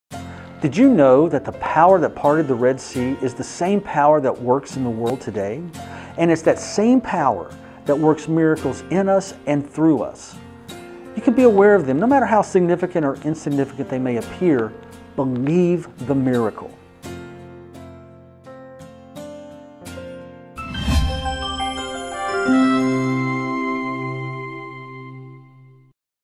The Miracles In You by Mark Victor Hansen
The power that parted the Red Sea is the same power that works in the world today. It’s the same power that works miracles in us and through us, no matter how significant (or insignificant) they appear at the time.

Believe the Miracle.